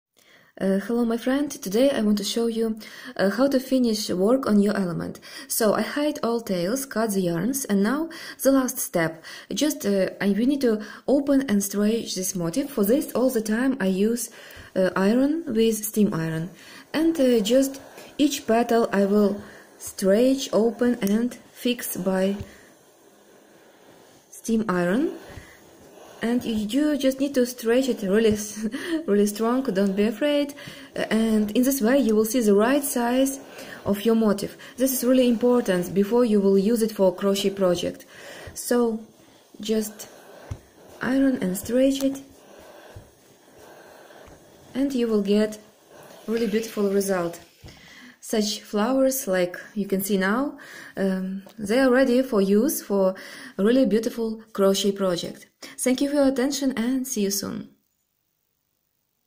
How to iron crochet motifs = tutorial for beginners
And even more, you will get access to 30 unique crochet courses about making beautiful motifs and laces.

Skype: yuliya.tushnytska